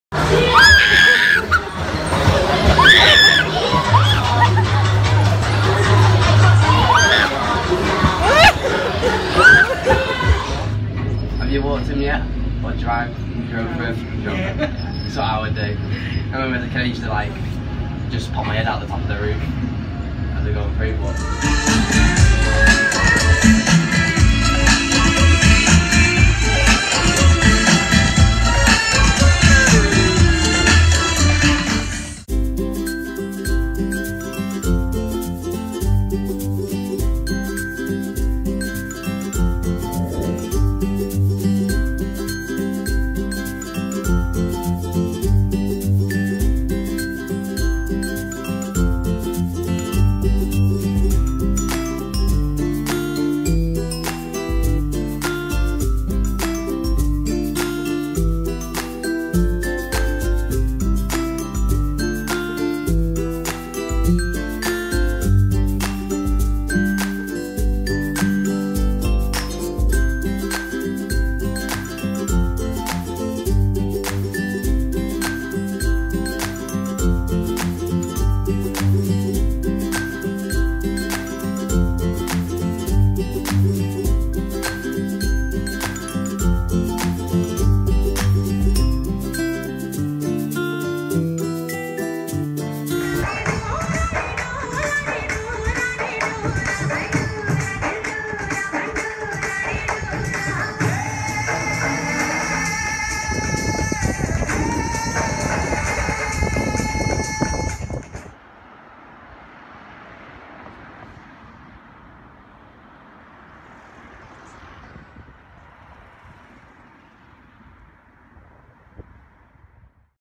Blackpool Illuminations 2016
We popped to Blackpool this week and although I didn't vlog I did shoot a couple of minutes of our evening in the lights.